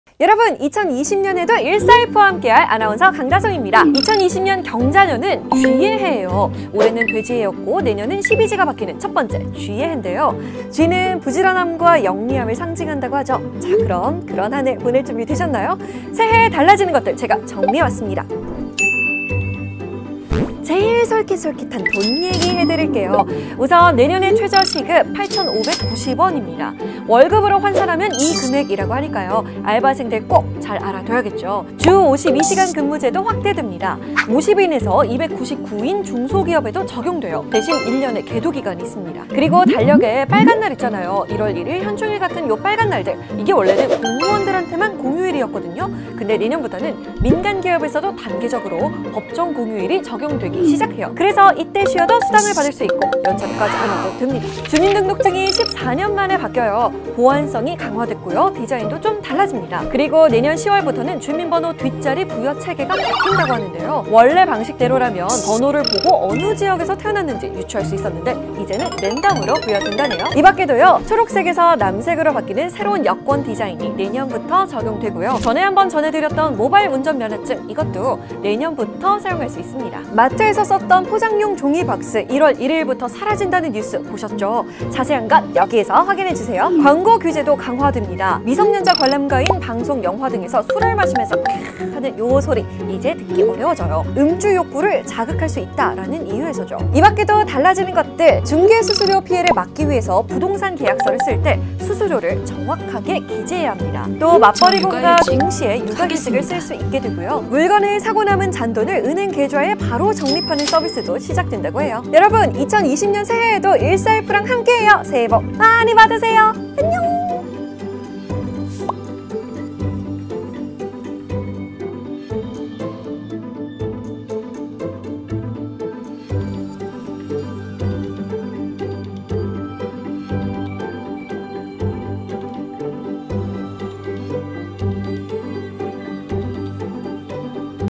내년에 주민등록증이 바뀐다고? 여권 디자인도? 새해에 달라지는 것들 / 14F
2020년 경자년은 쥐의 해! 부지런하고 영리한 한 해 보낼 준비 되셨나요? 내년에 바뀌는 최저임금은 얼마? 새로운 주민등록증과 여권은 어떤 거? 새해 달라지는 것들을 정리해 알려드립니다.